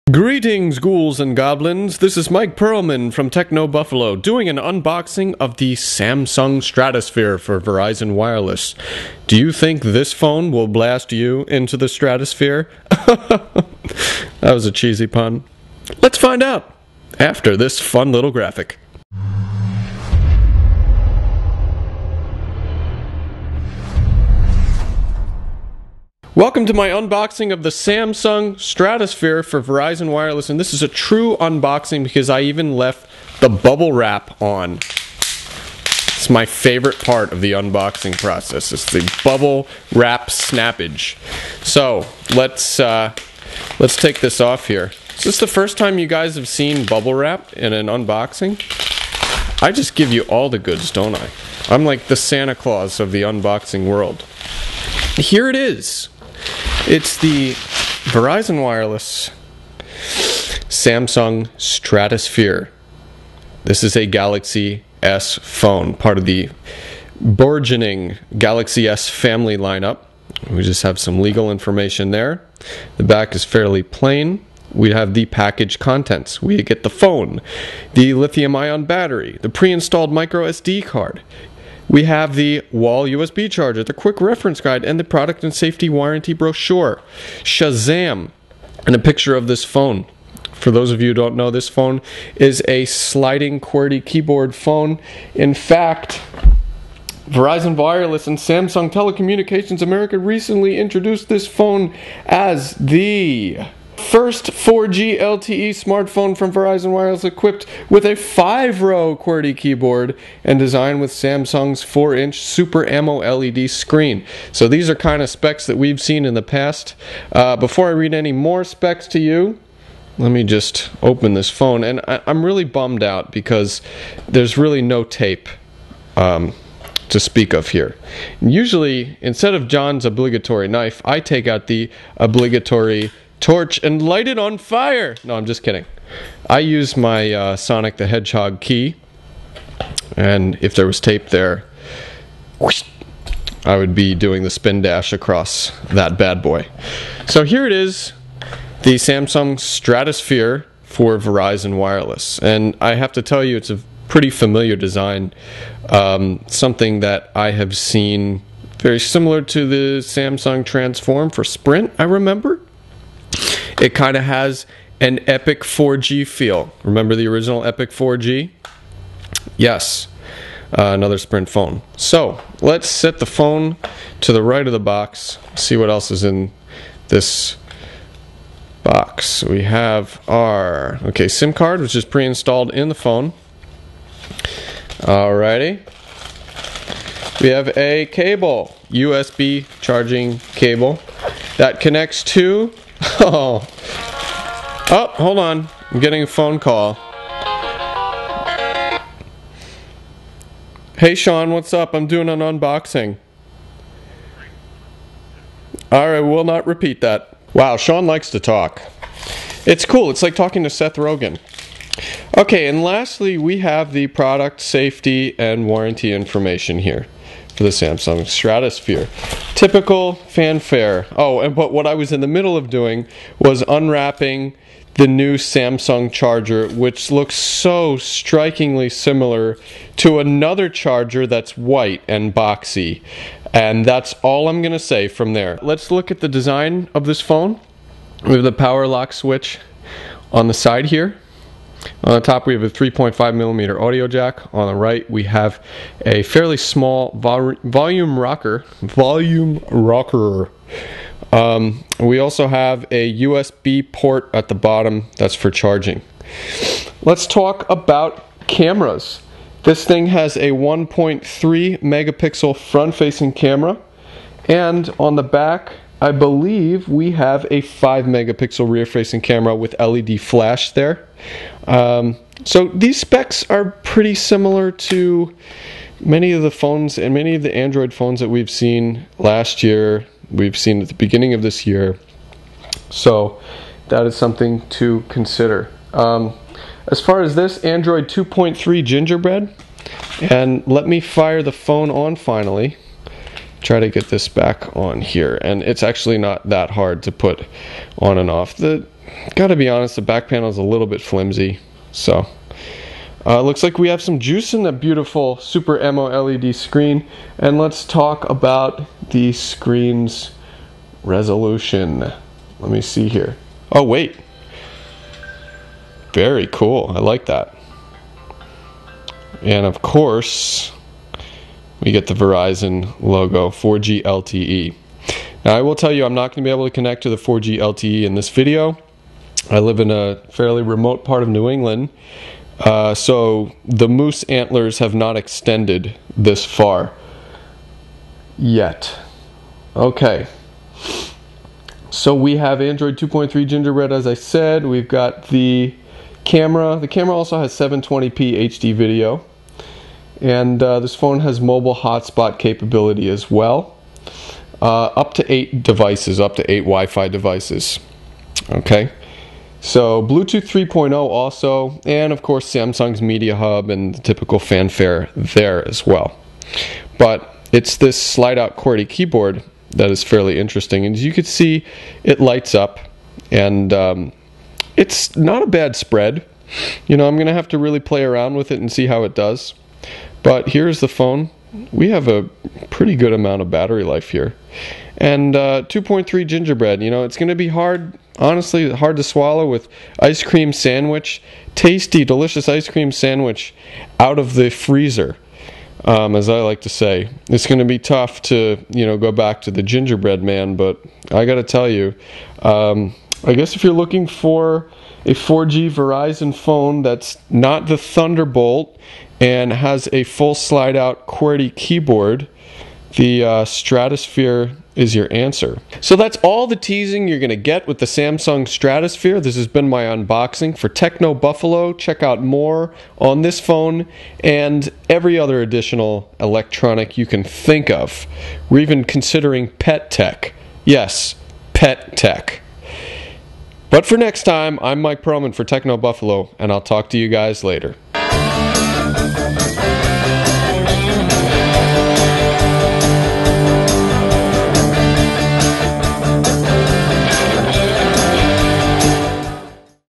Samsung Stratosphere Unboxing
Samsung's product map has been more like an Android assembly line lately, and the Samsung Stratosphere for Verizon Wireless is one of the more recent releases. The Stratosphere is billed as the "first 4G smartphone with a 5-row QWERTY keyboard," which is nothing to hold a parade over. The phone also flashes mid-range specs for this calendar year with a 1GHz Cortex Hummingbird processor, 512MB of RAM, 4-inch Super AMOLED screen and Android 2.3 Gingerbread. Is the Samsung Stratosphere merely last year's Epic 4G for this year's Verizon fleet, or does it have what it takes to tango with the Ice Cream Sandwich juggernauts of today? One would assume the former, but check out the full unboxing video to find out.
Samsung Stratosphere Key Features

        4G LTE -- 5 to 12 megabits per second (Mbps) and 2 to 5 Mbps in 4G LTE mobile broadband coverage areas
        Five-row QWERTY keyboard
        480 x 800 Samsung Super AMOLED™ Screen
        Android™ 2.3 Gingerbread
        Samsung 1 GHz Cortex A8 Hummingbird Application Processor
        Front-facing 1.3-megapixel camera for video chat and 5-megapixel rear-facing camera with autofocus and flash (720p HD playback and 480p DVD-quality recording)
        Mobile Hotspot Capability -- share 4G connection with up to eight Wi-Fi enabled devices or a 3G connection with up to five devices
        AllShare™ -- enables wireless connectivity through Digital Living Network Alliance® (DLNA)
        Samsung Media Hub -- Samsung's own content service, offering a vast lineup of critically acclaimed films and TV programs for rent or purchase
        Bluetooth® 3.0 technology -- support for headset, hands-free, stereo, phonebook access, and object push for vCard and vCalendar
        Multiple video file formats supported, including DivX and XviD
        Wi-Fi Connectivity (802.11 a/b/g/n)